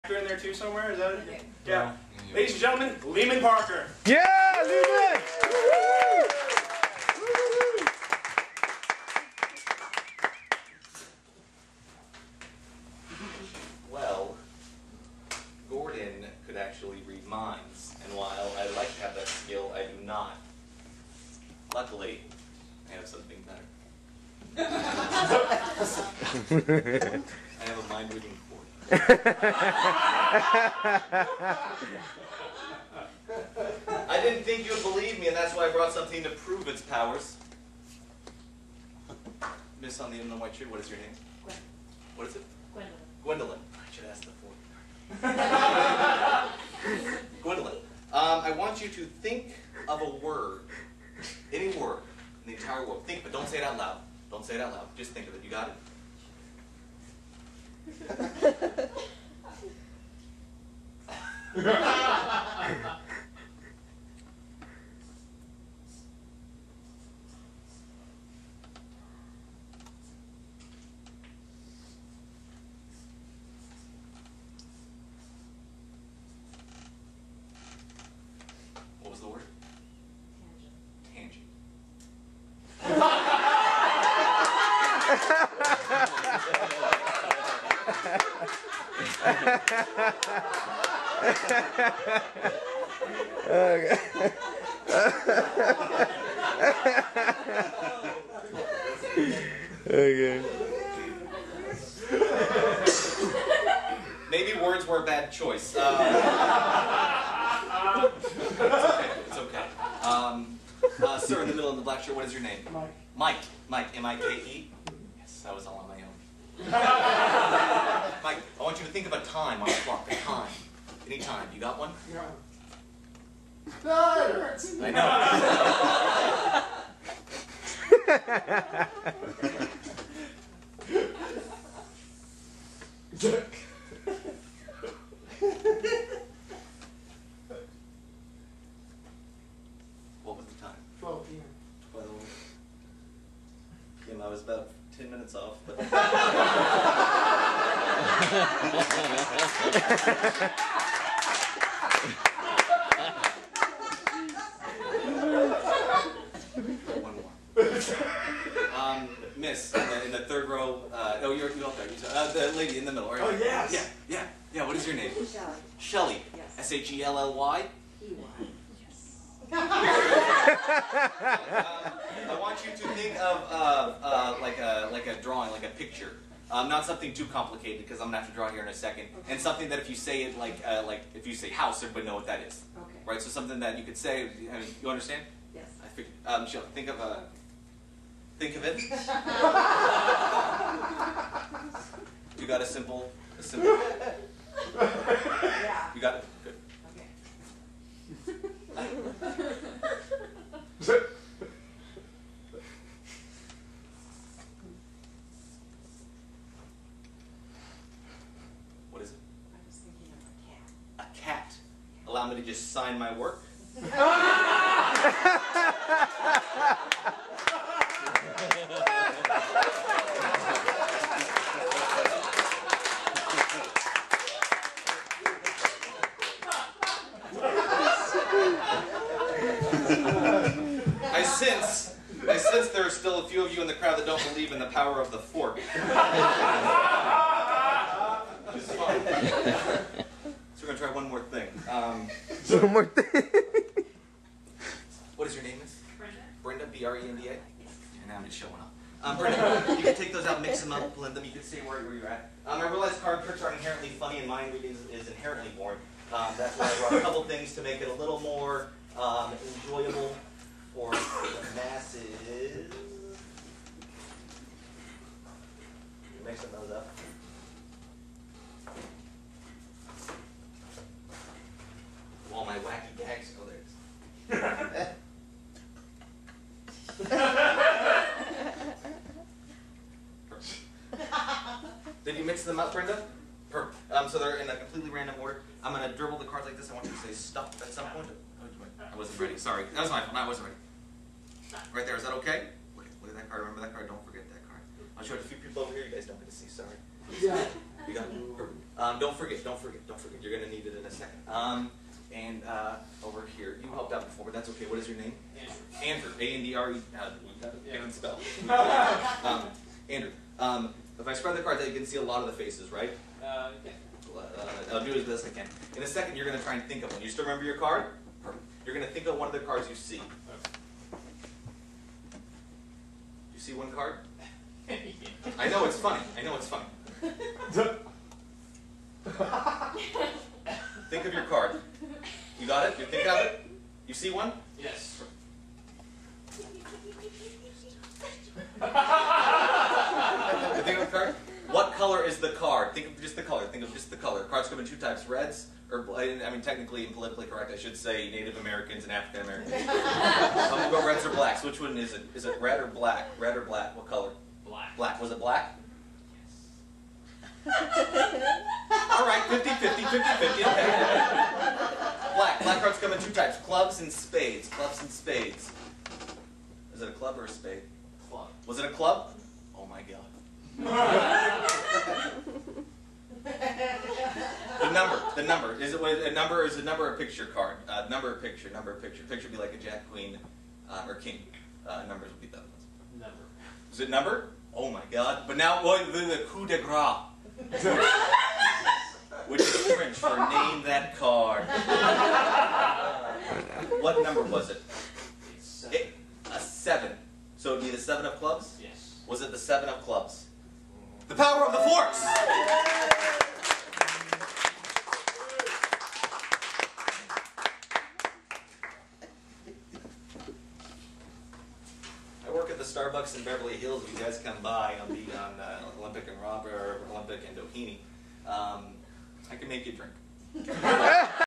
Mind Reading Fork
Come one, come all to see magician Leeman Parker demonstrate the power of the fork.